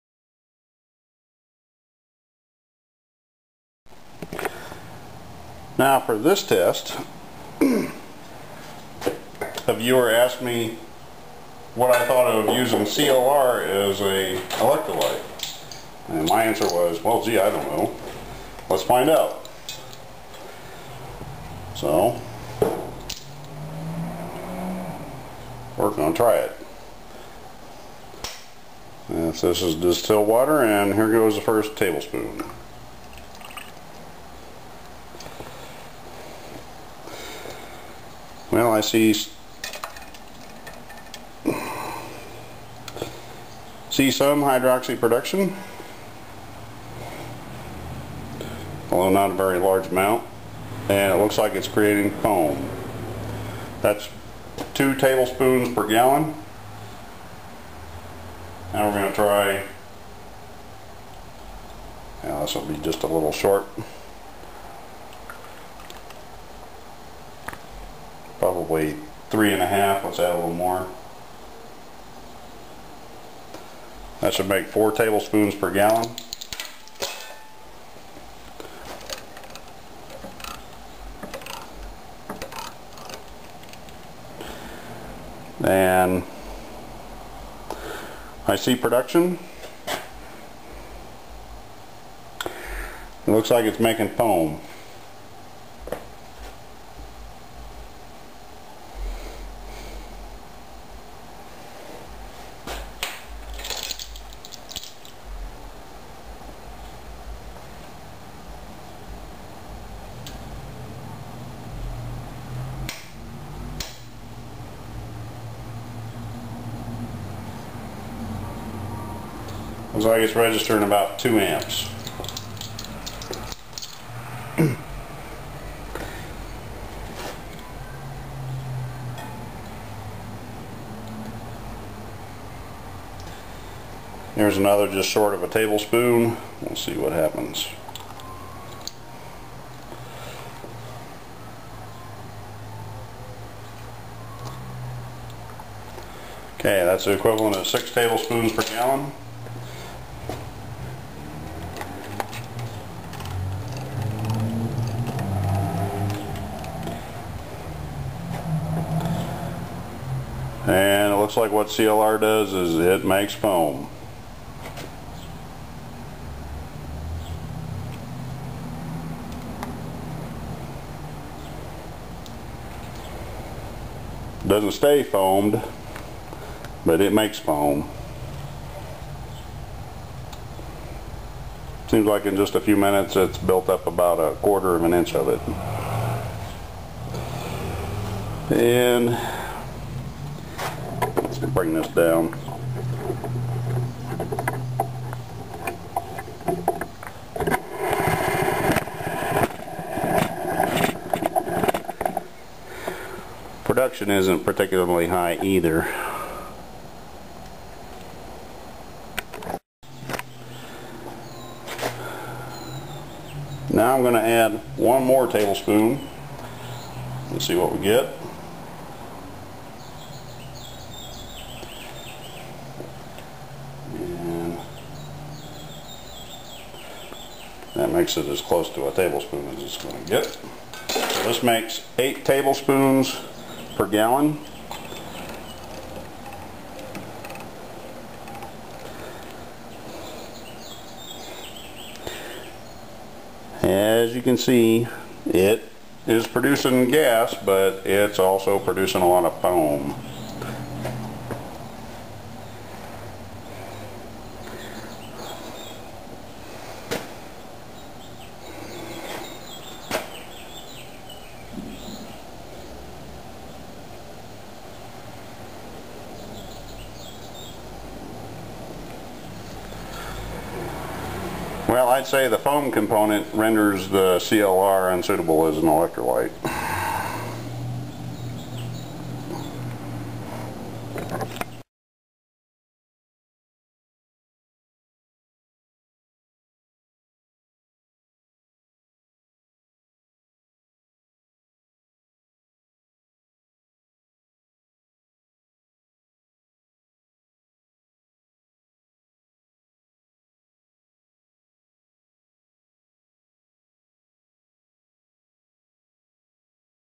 #08-032 Electrolyte Test Using CLR in Distilled Water
A viewer asked me about the possibility of using CLR as an electrolyte and I decide this was worth trying.  This video clip is the result.